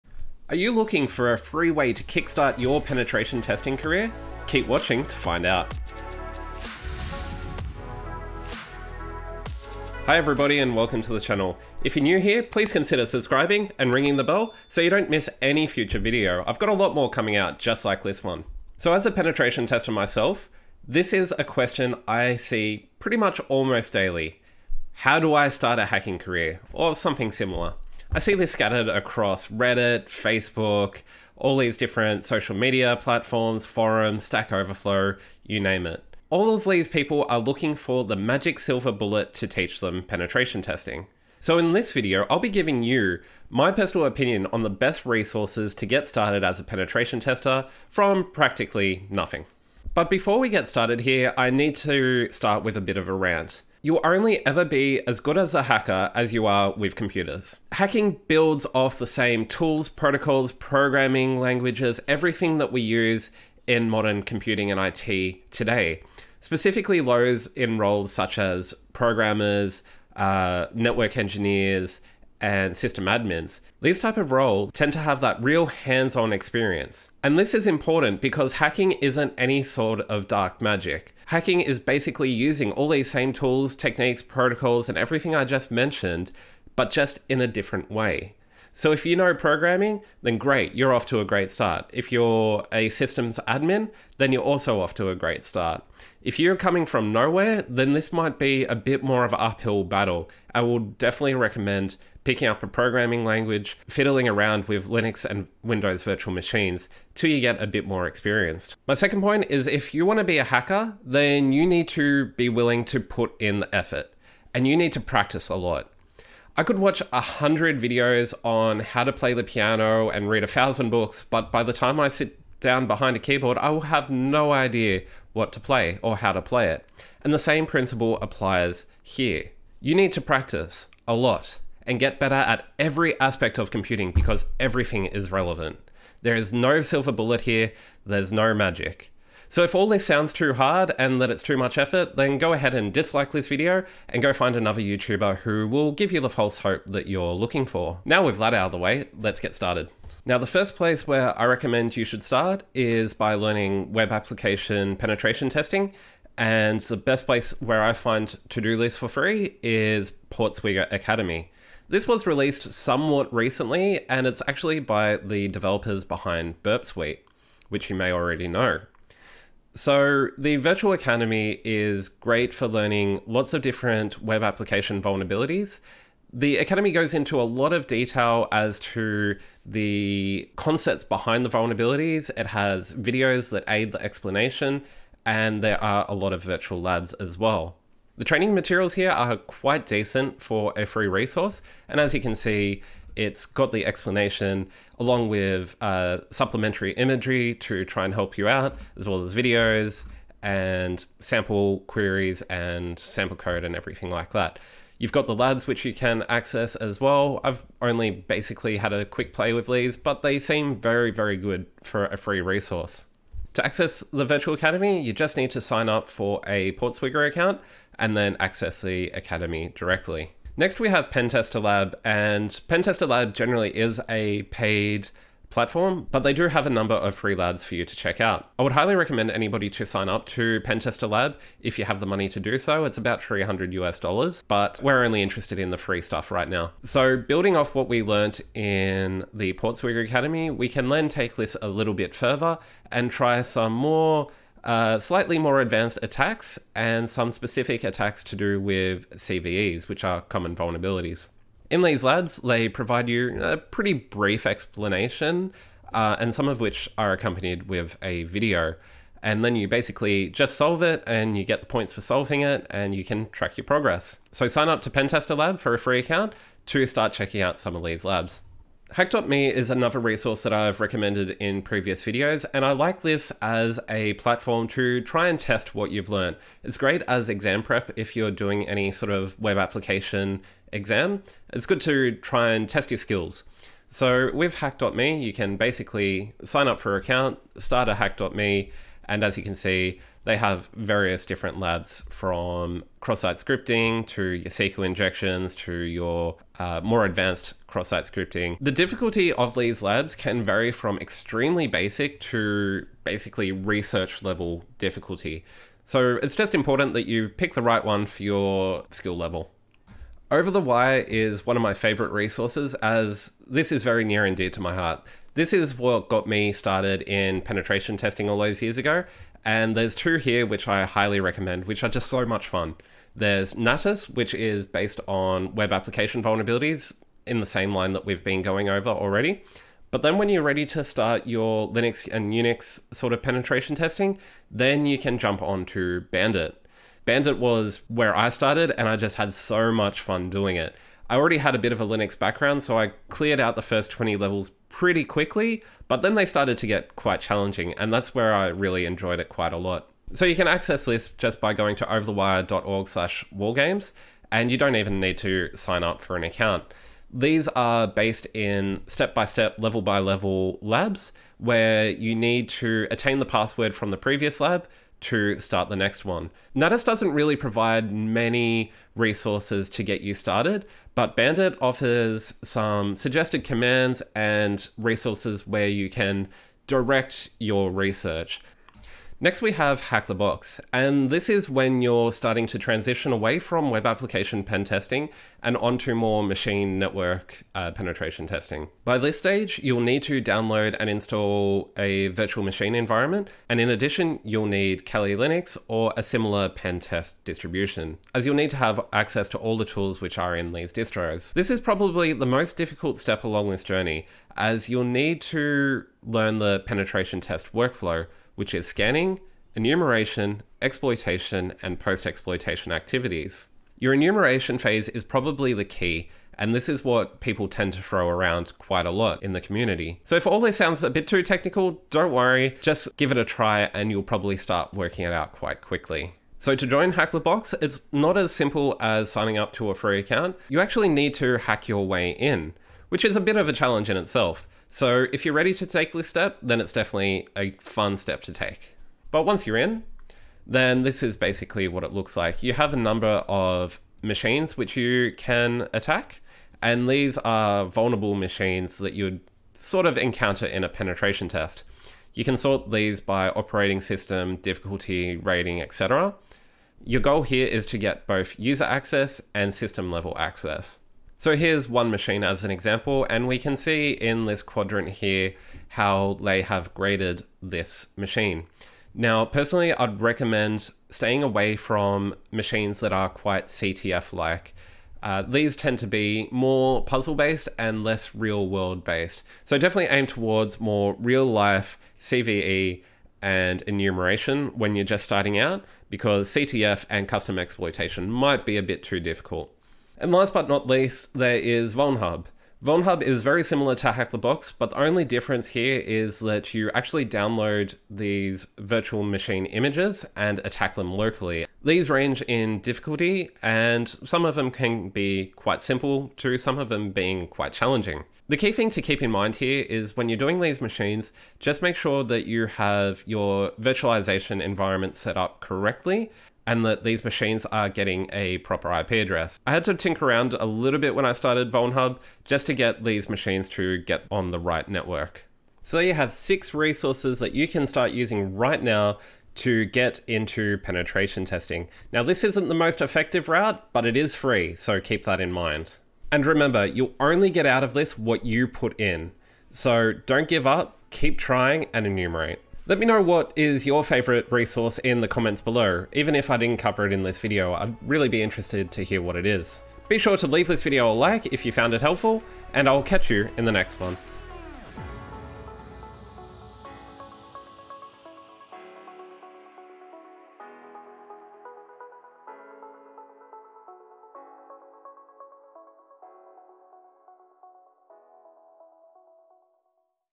6 Free Websites to Learn Hacking Fast!
This video will answer the eternal question of "How to learn hacking". All resources in this video are free, some with paid features. All of which are based on my personal experience and opinion and not sponsored content.

Happy Hacking